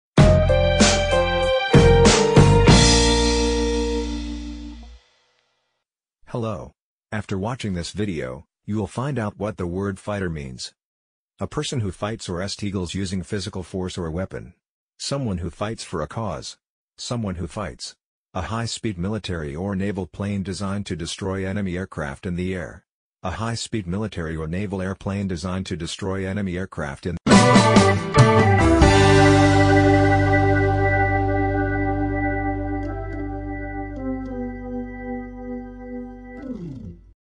Definition of the word "Fighter"
you will find full information about this word "fighter".
From the video you will learn the definition for the word fighter. The lexical meaning of the word fighter. What does the word fighter mean? And much, much more...